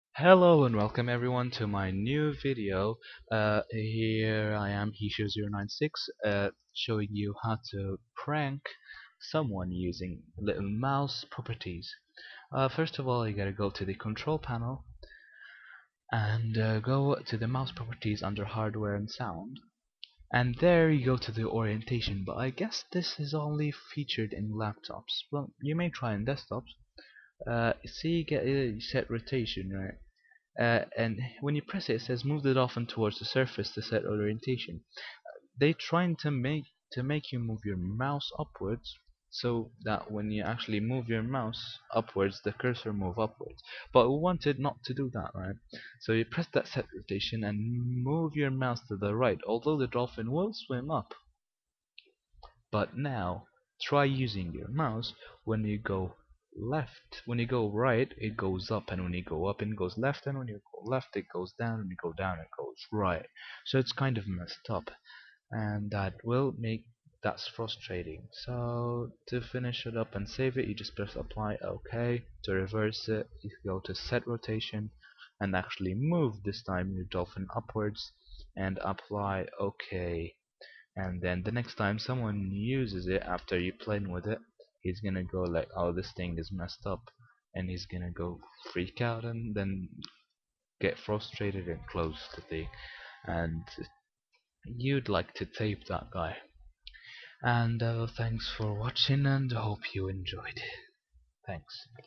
Mouse Prank (reverse cursor)
This prank is halrious when ur friends try they will be like, WTF is that this thing is messed up thnx for watching